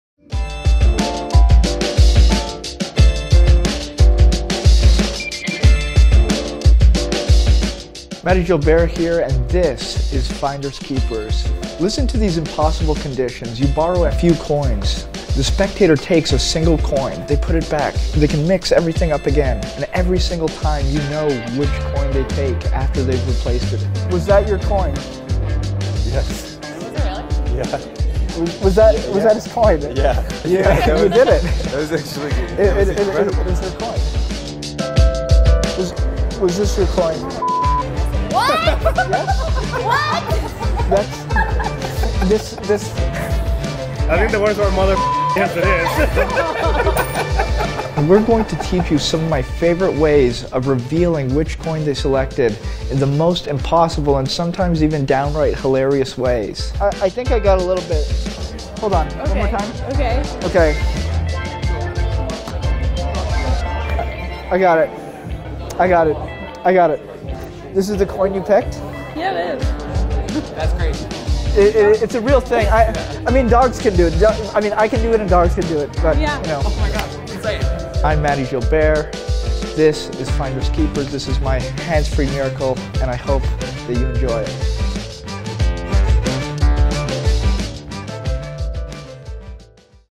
Finders Keepers by Mahdi Gilbert
"My inspiration" - David Blaine

"An extraordinary magician " - Derren Brown

"I believe Mahdi will be remembered in history as one of the great magicians" - Juan Tamariz

"The perfect magician" - Teller

"You are amazing!" - Dynamo

"You fooled us!" - Penn Jillette

Mahdi Gilbert is known for his beautiful thinking, and his inventive card work. With Finders Keepers he applies a fresh twist on a classic coin effect. In Finders Keepers the magician borrows a handful of change from a spectator. While the magician’s back is turned the spectator selects a coin and then mixes it back into the the rest of the change. Without any fishing or equivoke the magician is able to correctly identify the coin. The best part is there are no secret marks or gimmicks. All of the coins are actually borrowed. Finders Keepers is an impromptu miracle! Finders Keepers is easy to do, and Mahdi teaches not only how to do this effect, but also tips some great presentational hooks and motivations so that you too can get huge reactions. Once you find out the secret you’ll know this trick is a keeper.